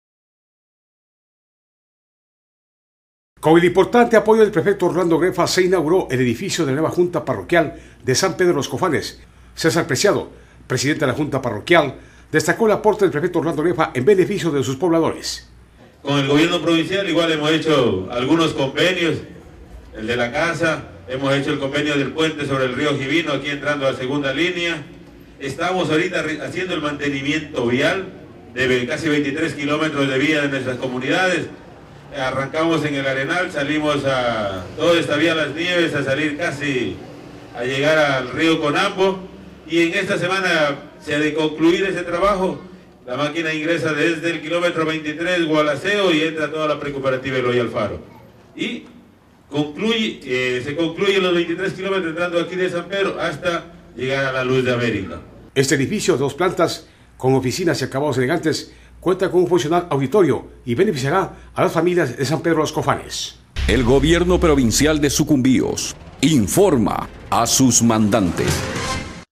PREFECTO ORLANDO GREFA INAUGURA NUEVO EDIFICIO PARA LA JUNTA PARROQUIAL DE SAN PEDRO DE
Informativo del Gobierno Autónomo Descentralizado de la Provincia de Sucumbíos